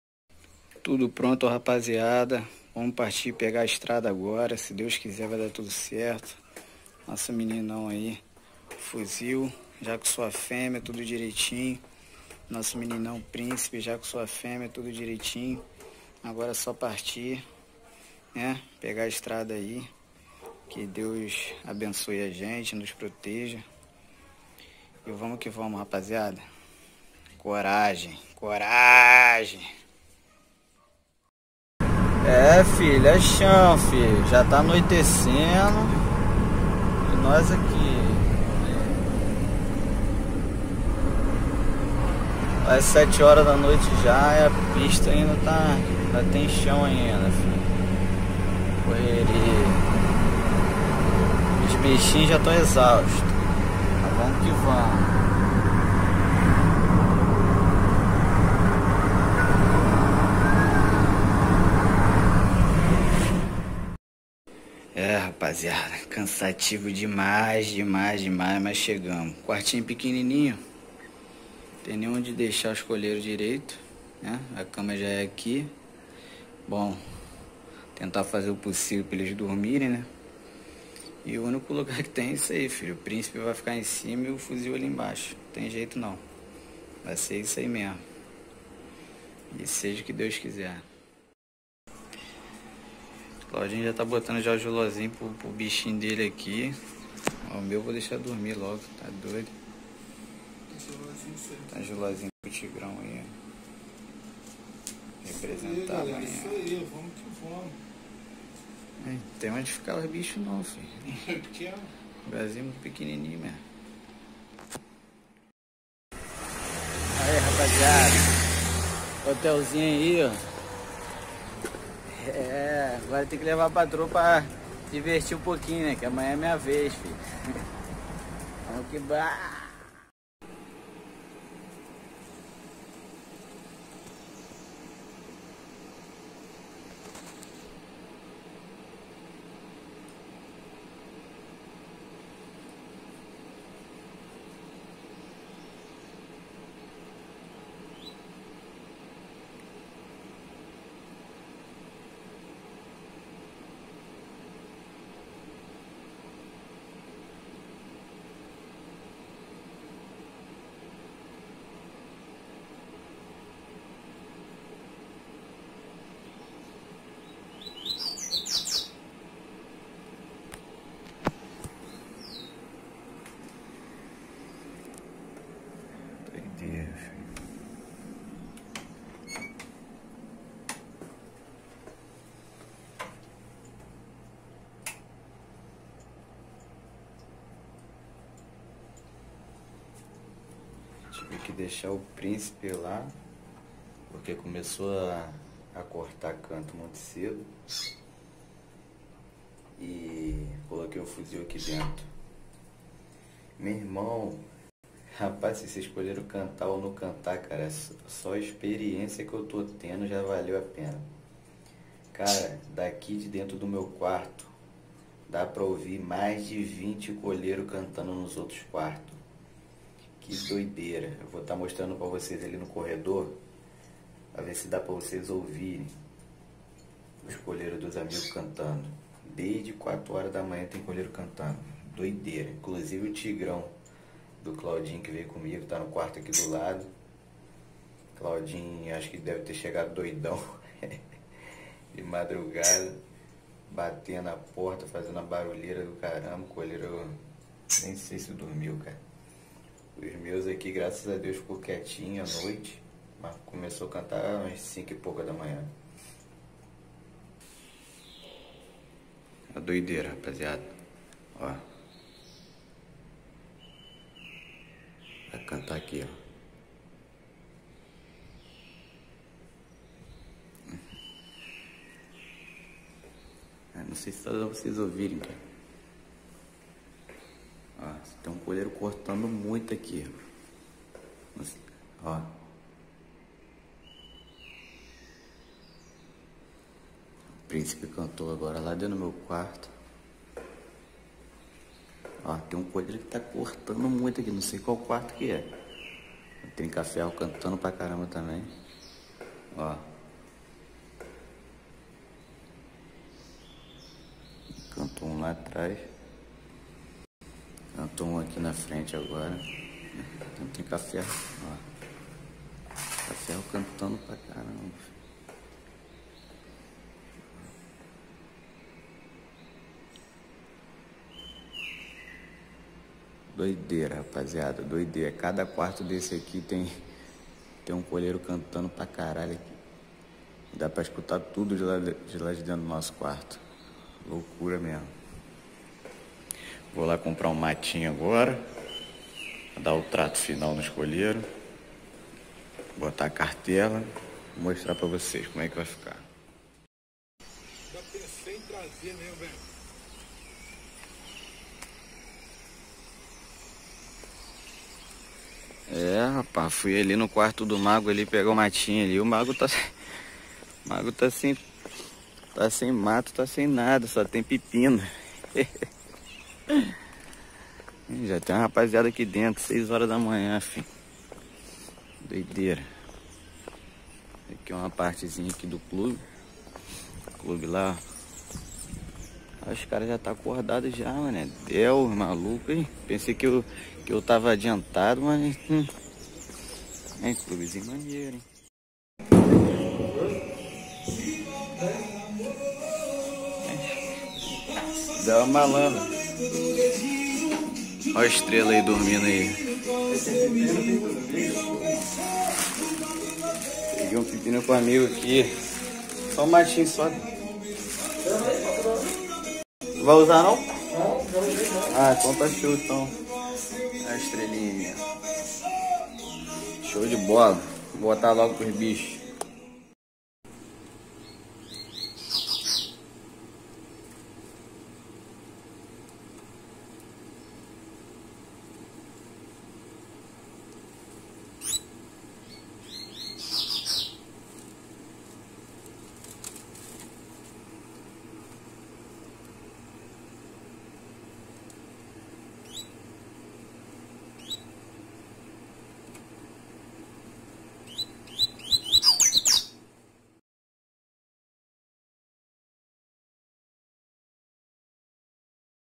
TORNEIO NACIONAL DE COLEIRO E PAPACAPIM EM BARRA MANSA, VIAGEM CANSATIVA MAS MUITO BACANA PARTE 1
Mais tarde lanço a outra parte rapaziada